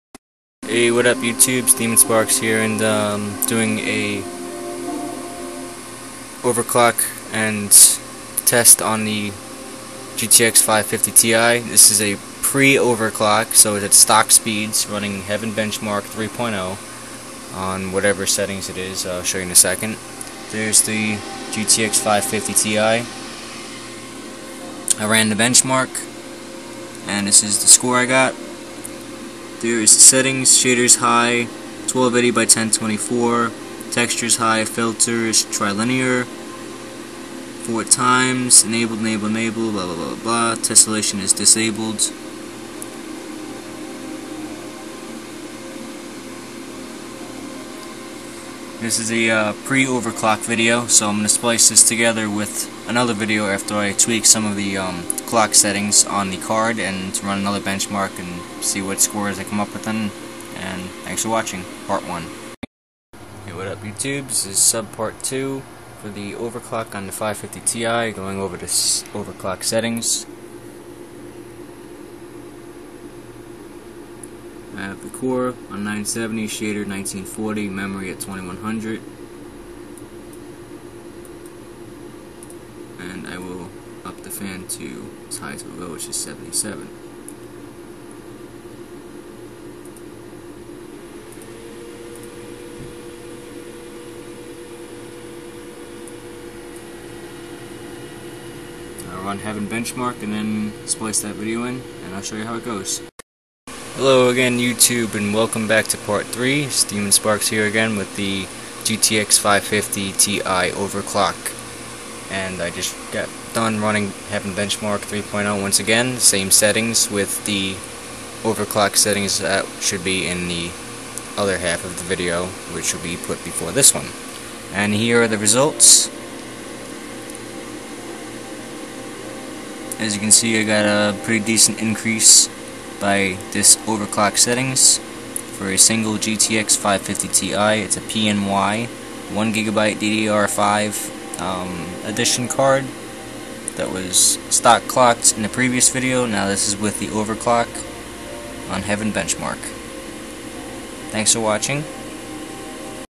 GTX 550Ti benchmark and overclocking
part 1 pre overclock benchmark score.
part 2 overclock settings.
part 3 benchmark with overclock on same settings.

ASRock 970 EXTREME3 mobo
AMD Athlon II 640 quad core 3Ghz
Corsair CX600 PSU
2x2Gb G.Skill Ripjaw 1600OC DDR3 RAM
PNY GTX 550Ti 1Gb GPU
Toshiba 5400Rpm 160Gb HDD primary
Seagate Baracuda 7200 500gb Storage/slave